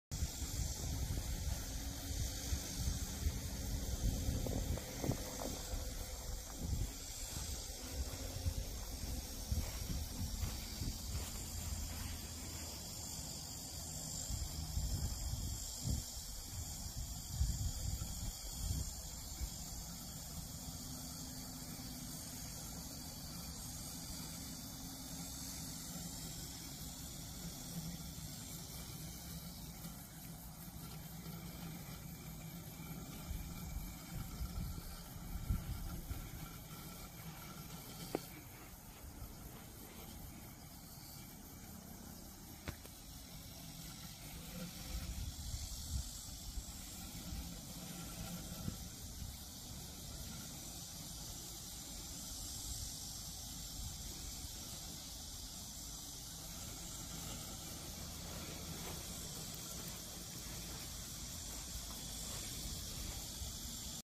태안 그람피하우스로 분양된 허스크바나 로봇 잔디깎기 오토모어 430X
허스크바나의 오토모어는 다른 로봇 제품보다 잘 안 알려진 장점이 많은 똑똑한 모어입니다. 상세한 정보는 우창통상의 허스크바나 전문 네이버스토어에서 찾아 보세요.